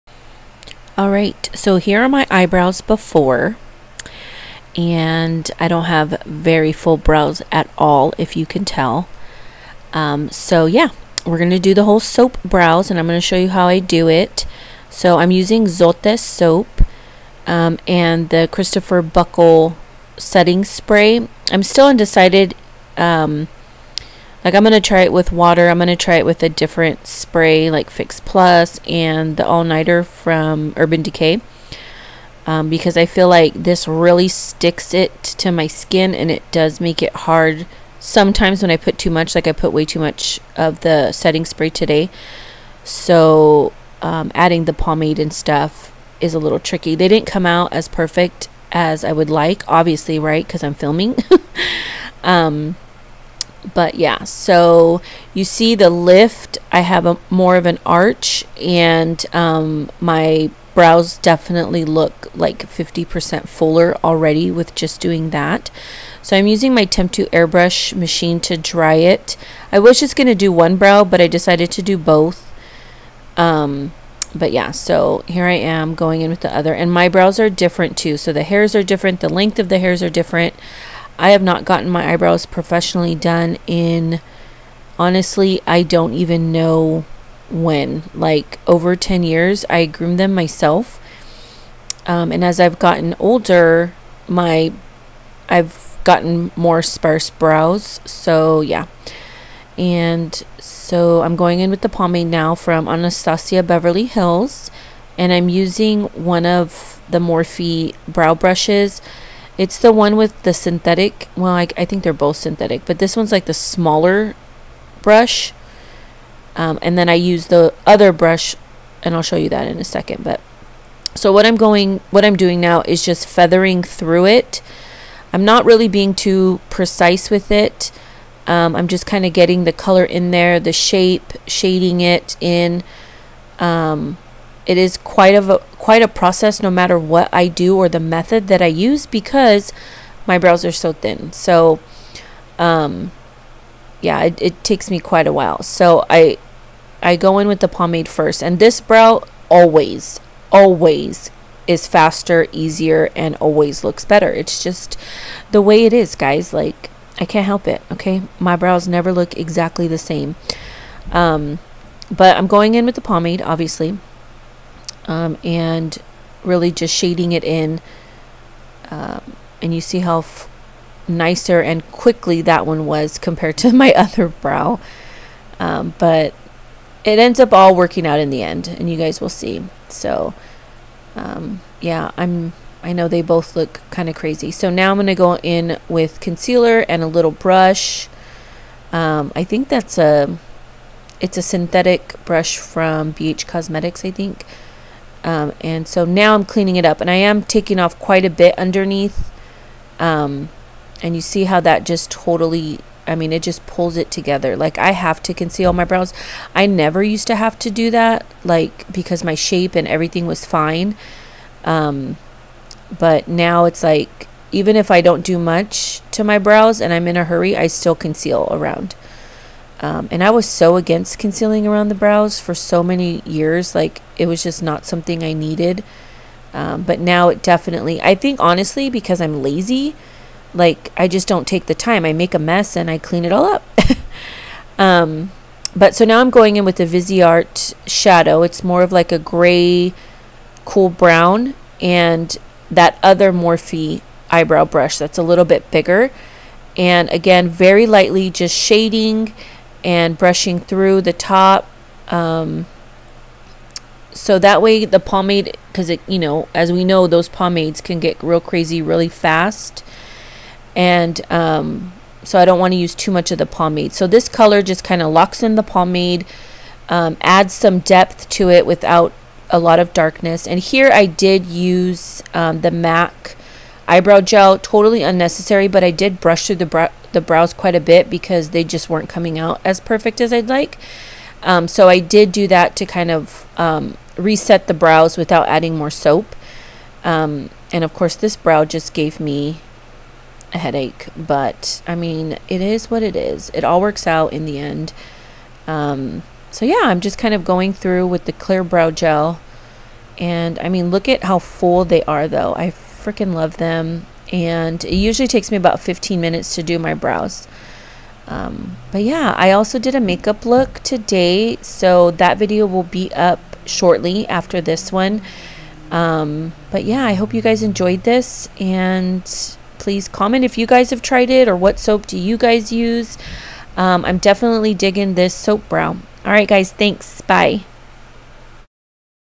Zote Soap Brows...On thin hair
Still new to this technique so bare with me lol!! Over all it’s great and I’ll continue to do it! I do like a fuller brow so this helps me achieve that.

Xoxo
Melissa